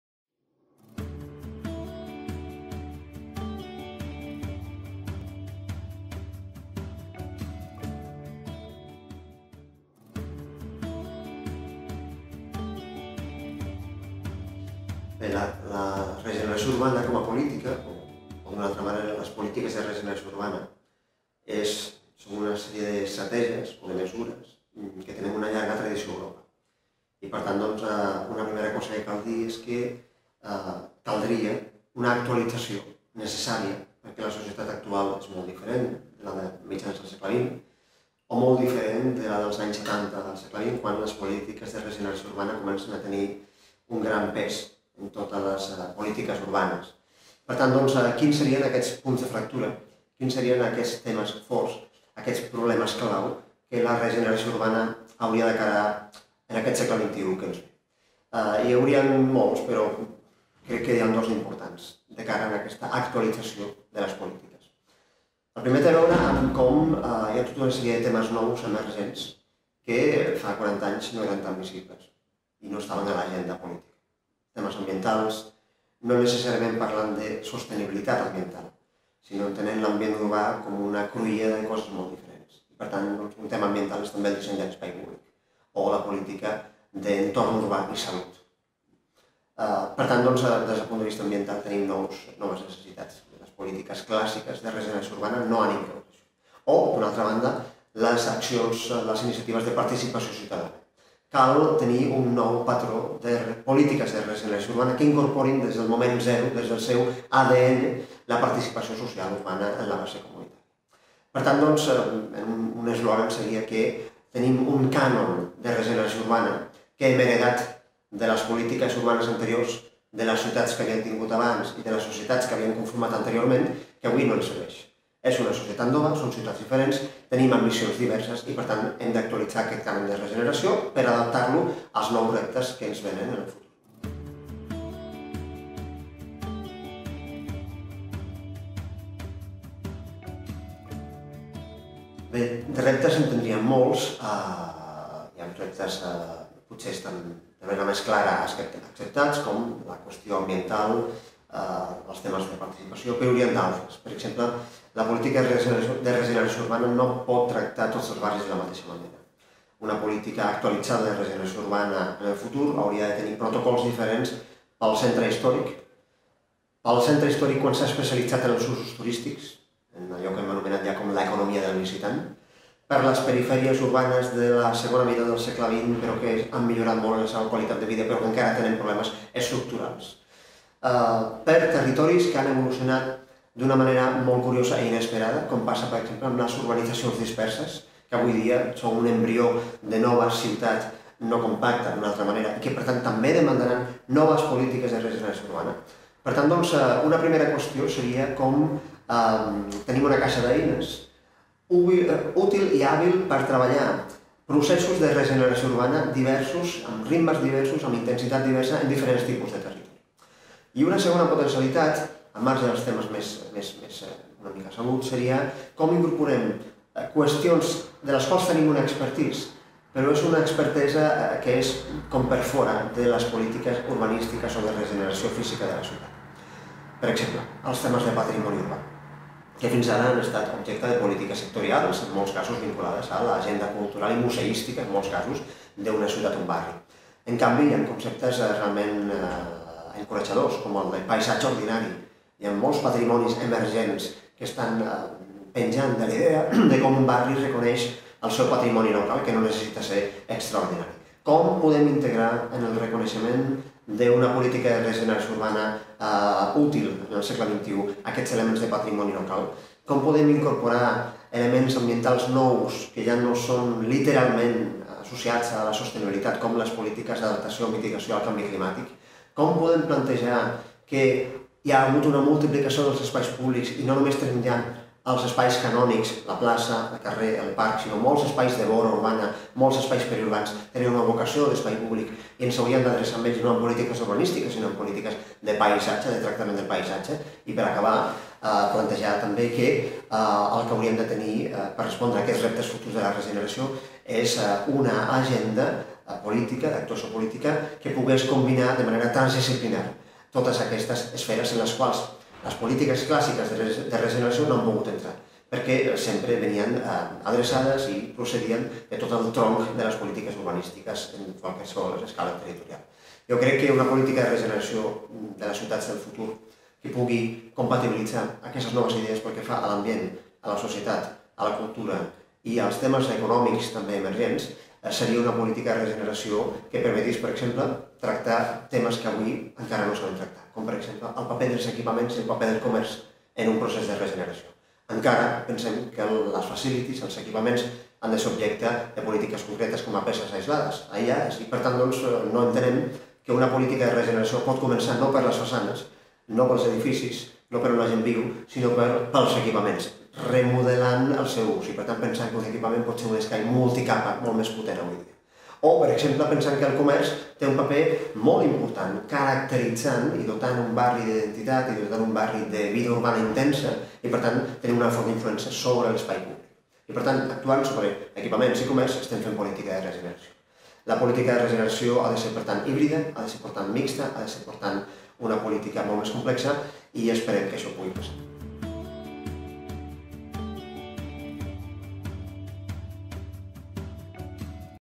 El Consell Assessor del Pla de Barris: Francesc Muñoz (Regeneració urbana)
"Transformar la ciutat amb la ciutadania: Criteris i reflexions per al Pla de Barris de Barcelona. Consell Assessor del Pla de Barris". Francesc Muñoz (Regeneració urbana)